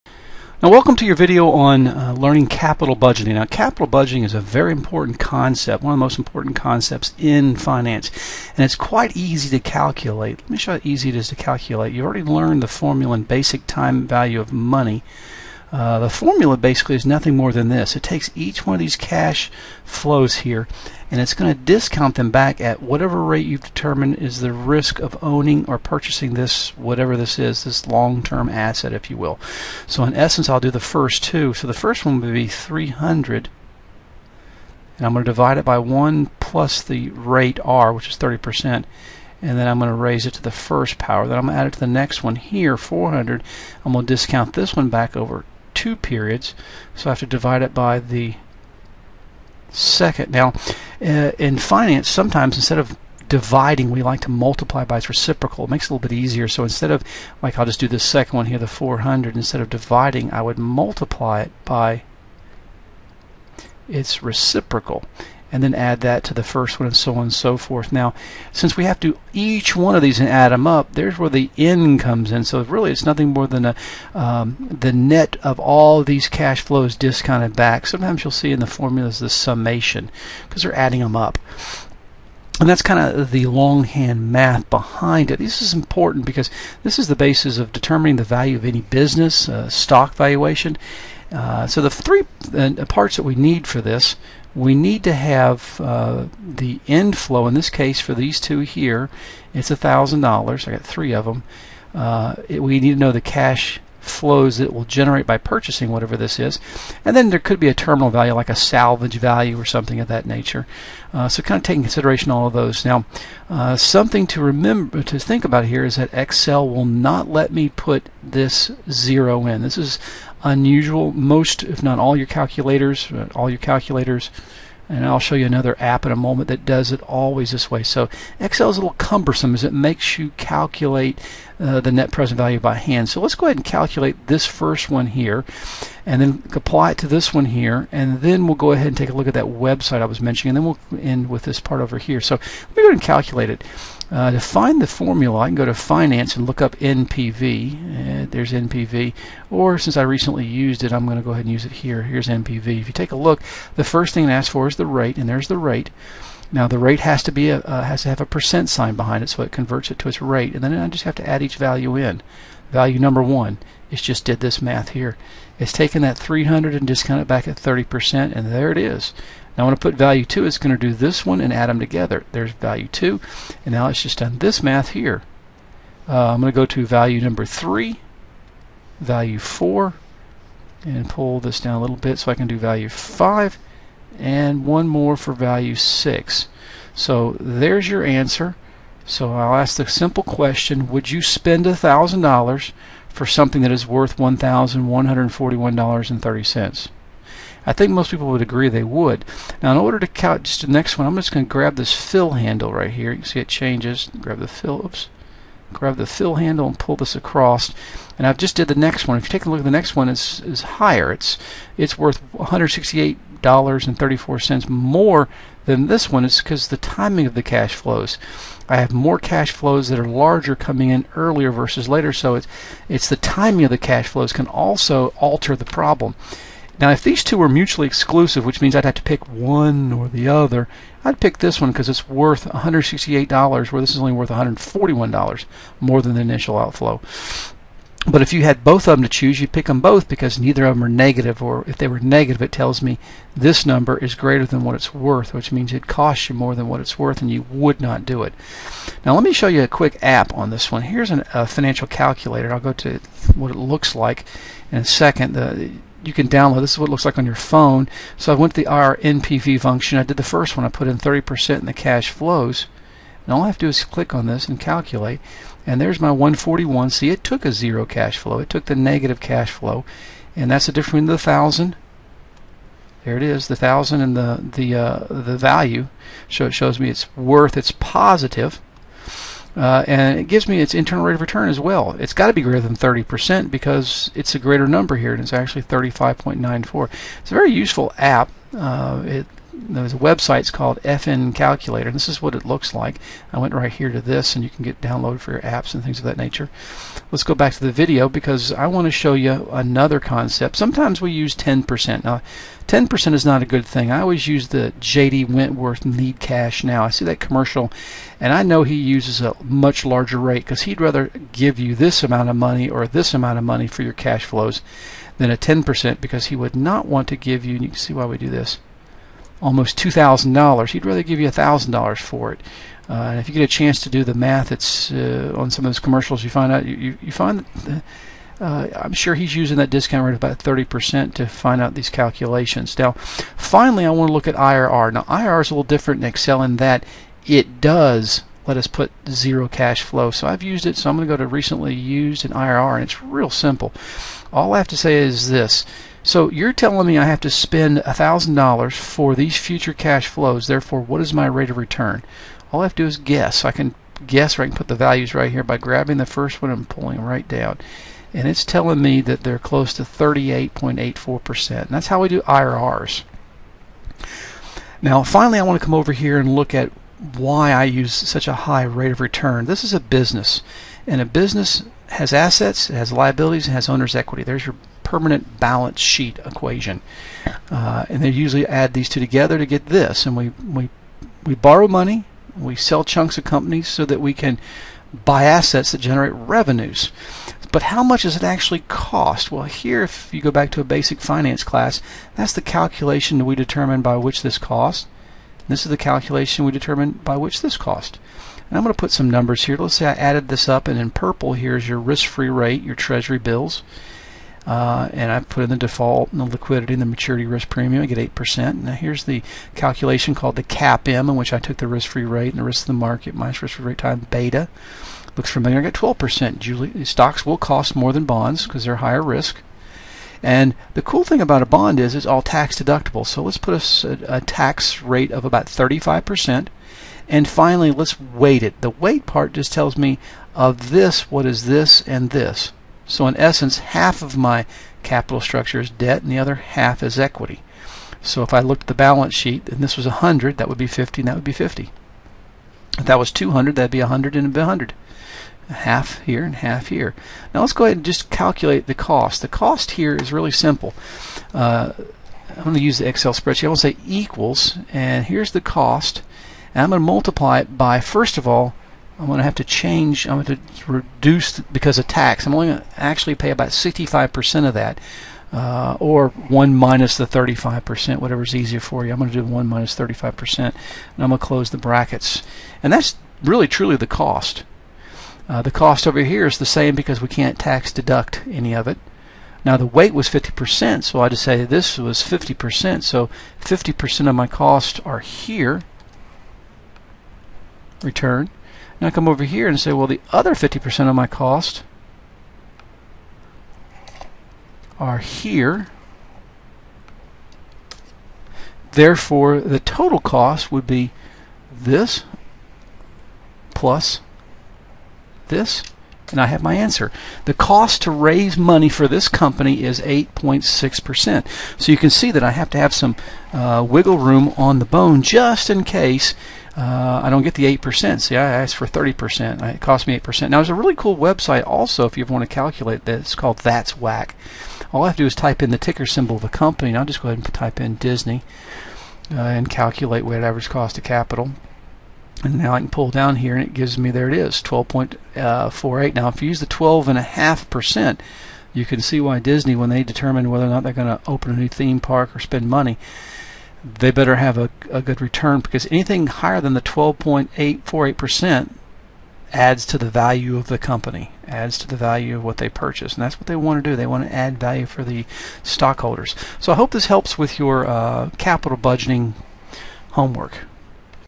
Capital Budgeting
Capital Budgeting and Internal Rate of Return using Ez Calculator aka fncalculator.com and Excel.